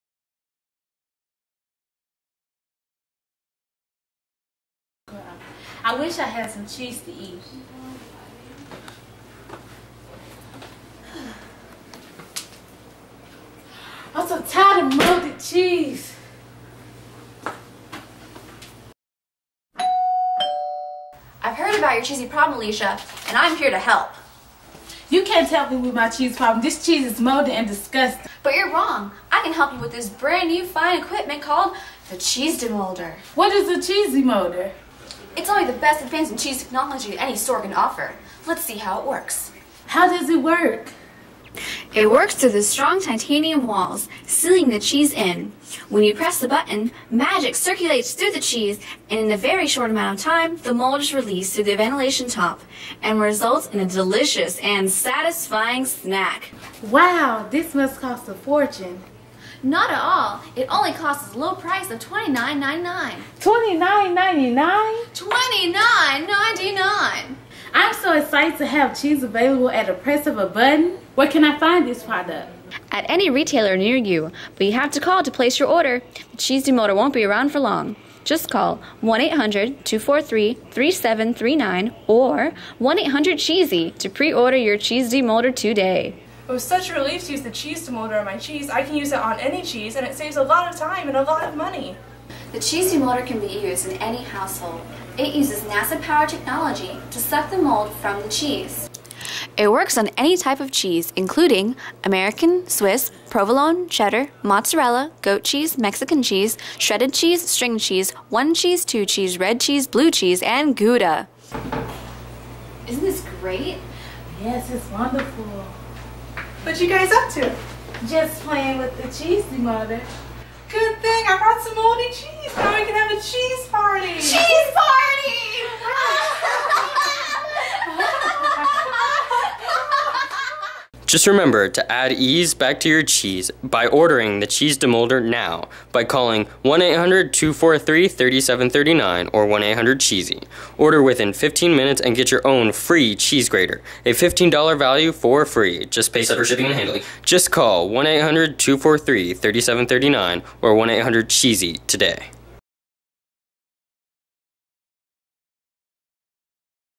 Cheese demolder
A Class project, the product was not my idea, I did the editing and final voice over at the end